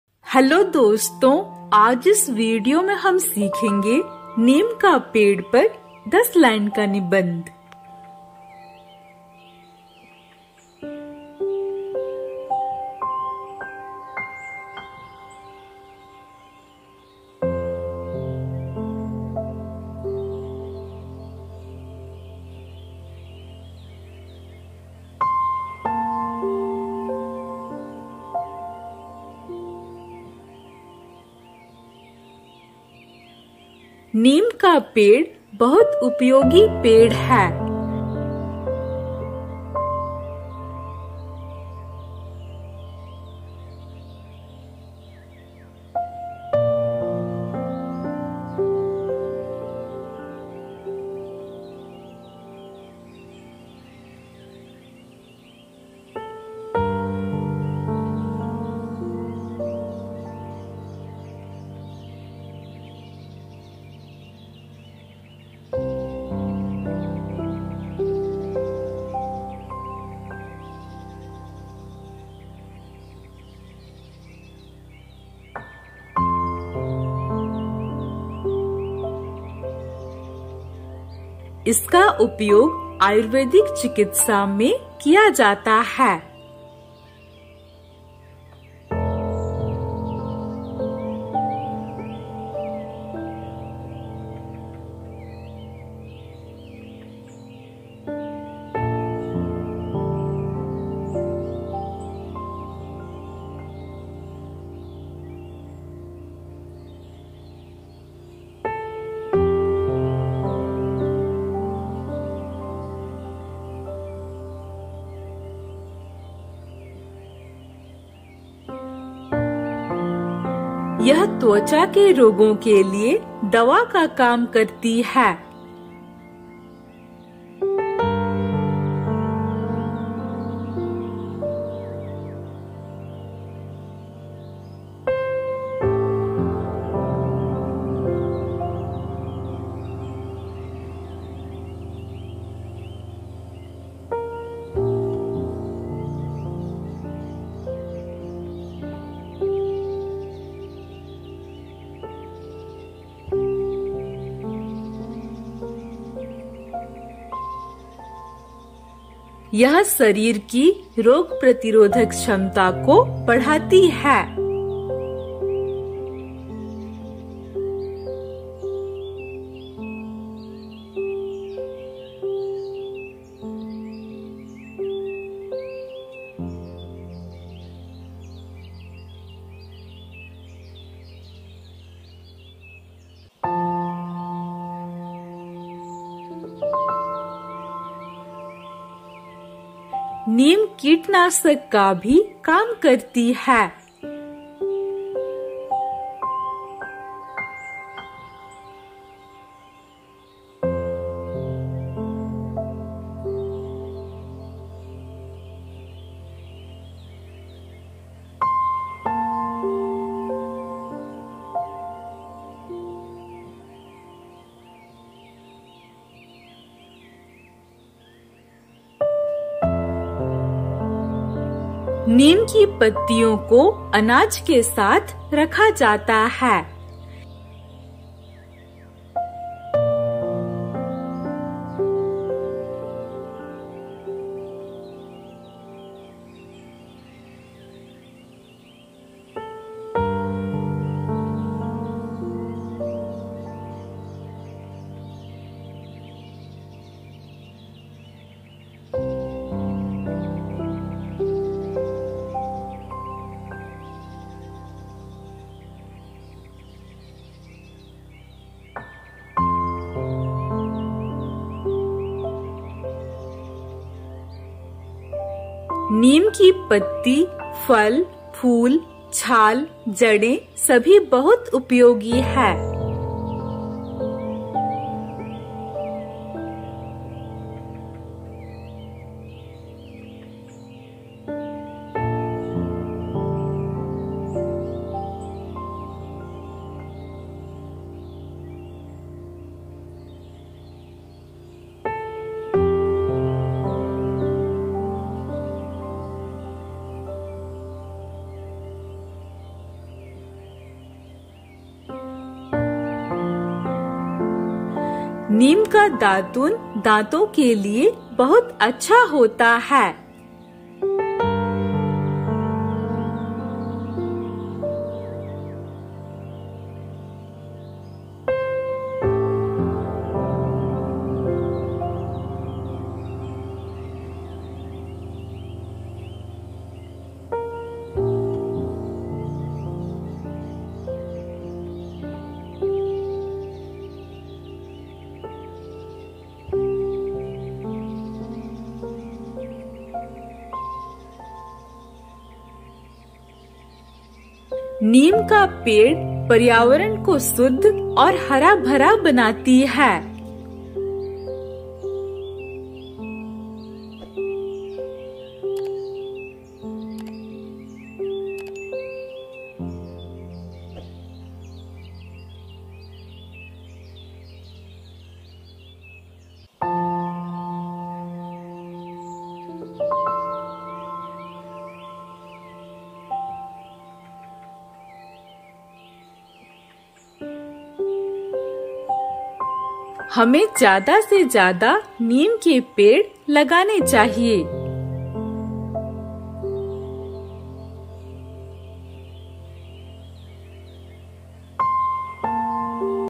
10 lines Essay on Neem tree in Hindi | नीम का पेड़ पर निबंध 10 लाइन | Neem ka ped par Nibandh
10 lines Essay on Neem tree in Hindi | नीम का पेड़ पर निबंध 10 लाइन | Neem ka ped par Nibandh

This video is about 10 lines on Neem tree essay in Hindi. If you want to write an essay on Neem tree in Hindi or "Neem ka ped per nibandh" then this video will be helpful to you.